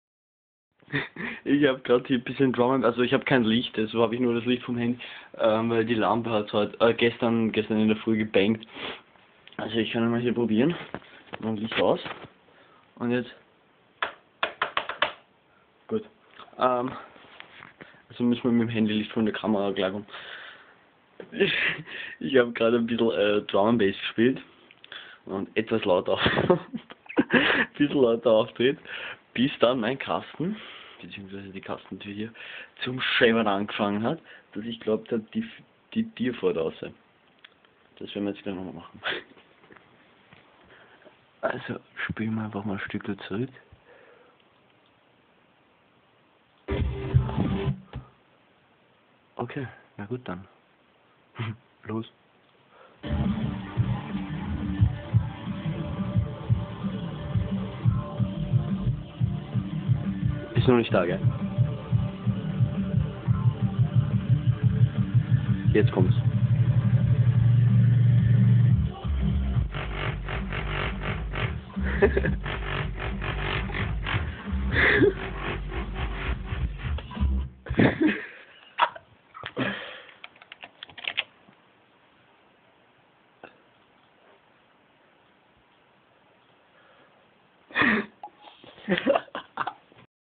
Bass aufgedreht kasten fällt auseinander xD
this channel is deprecated. new videos/streams on this channel jo ich hab meine 1400 watt mal probiert... da is mir halbert der kasten auseinander geflogen xDDDD